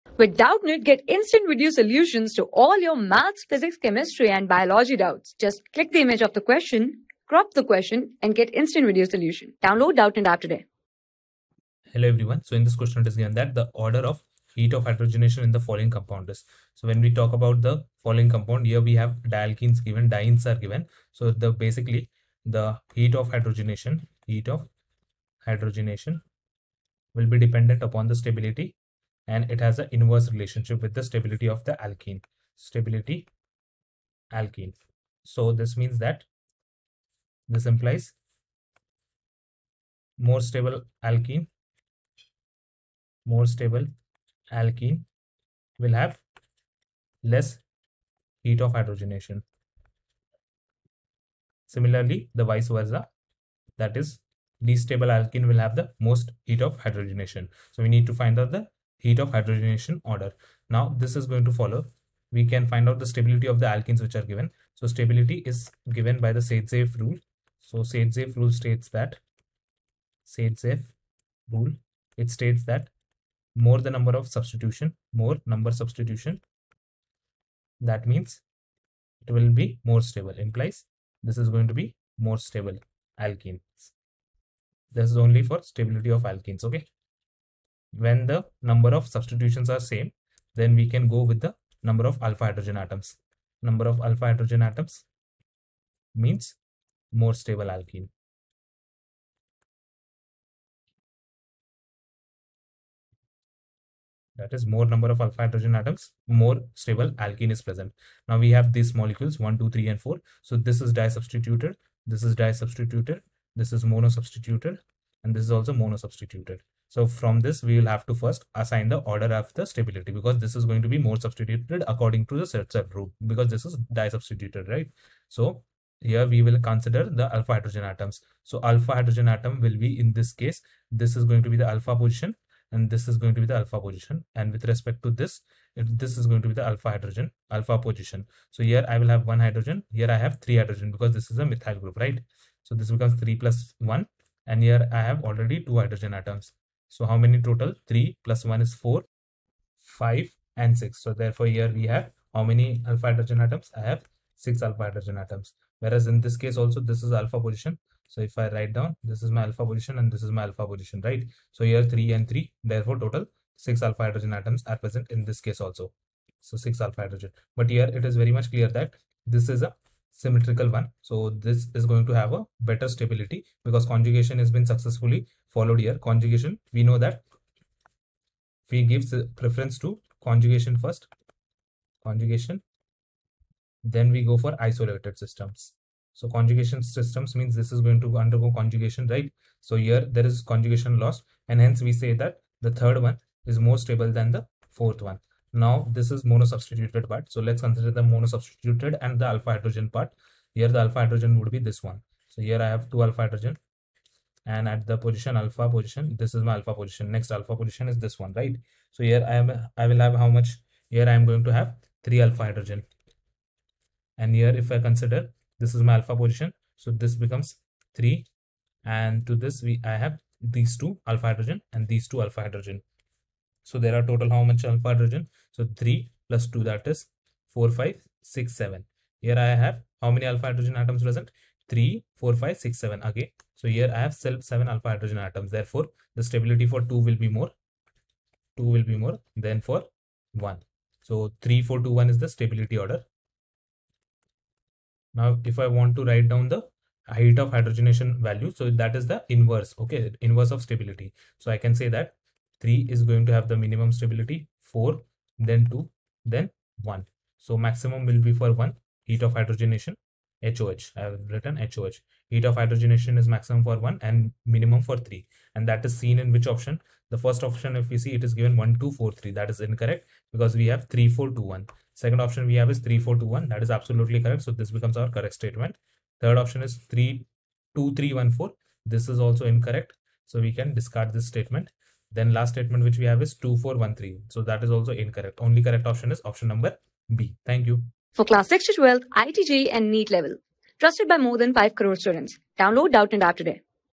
The order of heat of hydrogenation in following compounds is: | 11 | GENERAL ORGANIC CHEMISTRY-...
The order of heat of hydrogenation in following compounds is:
Class: 11
Subject: CHEMISTRY
Chapter: GENERAL ORGANIC CHEMISTRY-I
Board:IIT JEE

👉 Have Any Query? Ask Us.